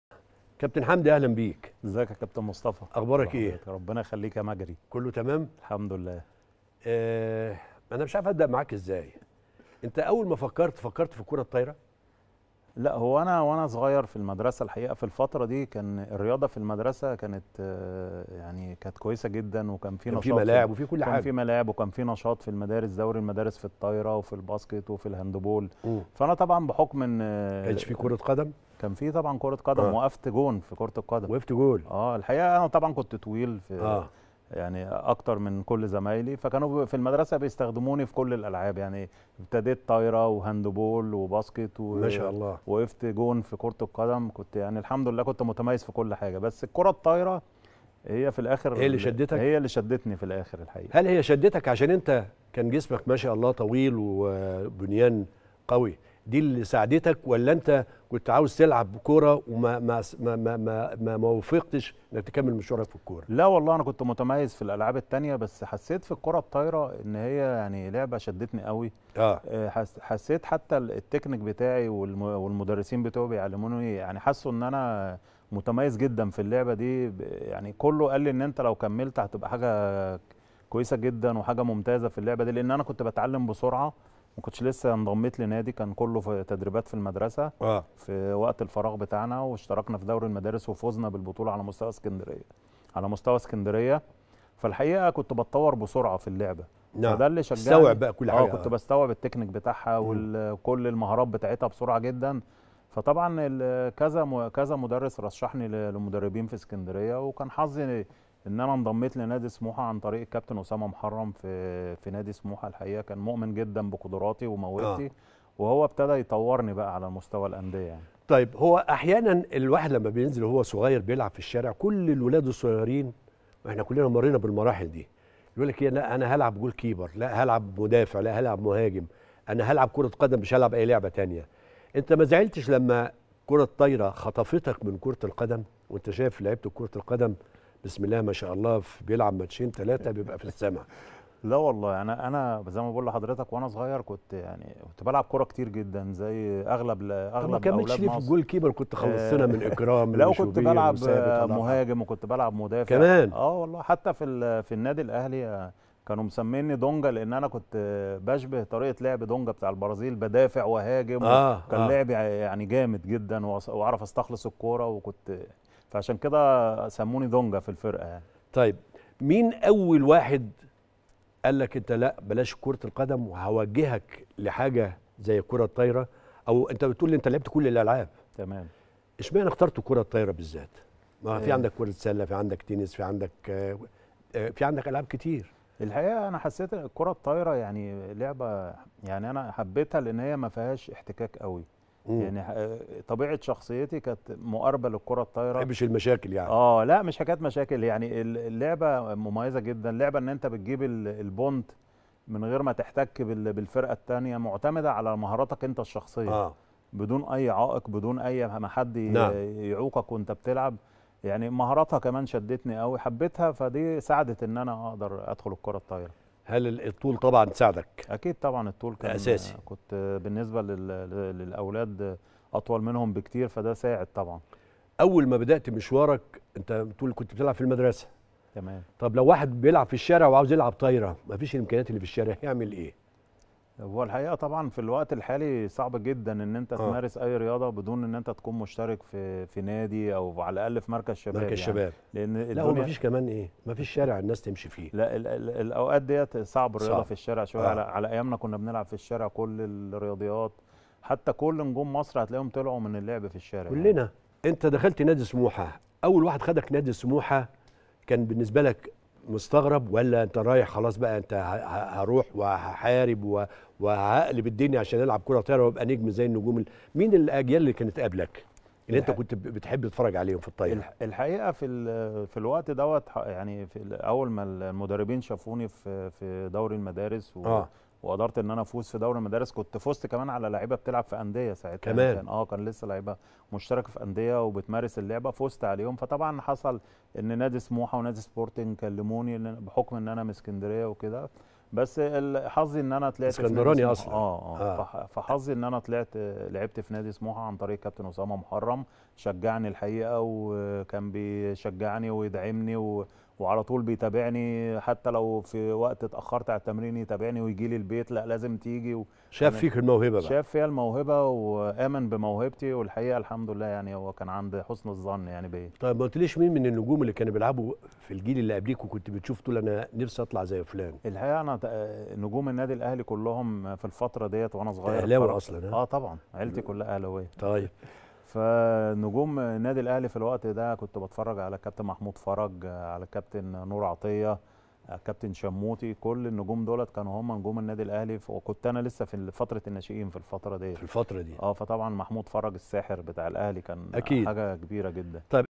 حمدي الصافي يوضح كيف توجه حبه إلى الكرة الطائرة رغم الاشادة به في كرة القدم في العديد من المراكز
الكابتن حمدي الصافي يوضح كيف توجه حبه إلى الكرة الطائرة رغم الاشادة به في كرة القدم في العديد من المراكز | المجري